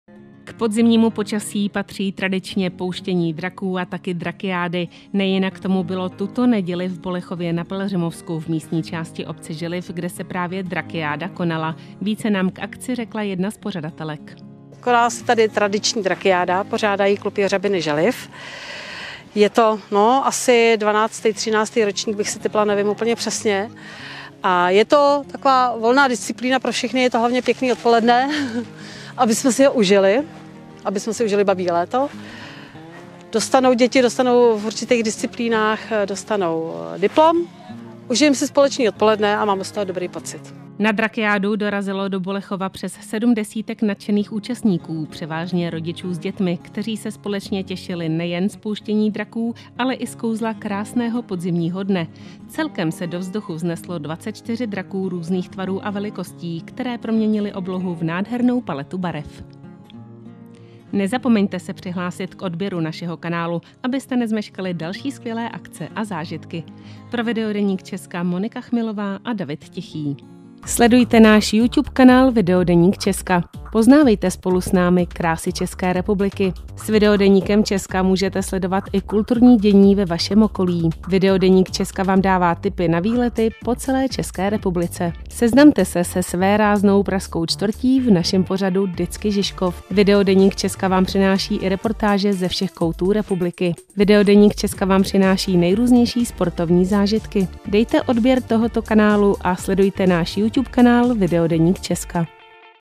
Drakiáda Želiv 2024
Vítejte na našem kanálu! Přinášíme vám záznam z úžasné akce Drakiáda, která se konala v malebné obci Želiv na Pelhřimovsku. Vychutnejte si kouzlo krásného podzimního dne, kdy počasí skutečně přálo a babí léto ukázalo svou nejlepší tvář. Na akci dorazilo přes sedm desítek nadšených účastníků, převážně rodičů s dětmi, kteří se společně těšili z pouštění draků. Celkem se do vzduchu vzneslo 24 draků různých tvarů a barev, které proměnily oblohu v nádhernou paletu barev. Připojte se k nám a zažijte atmosféru této jedinečné události!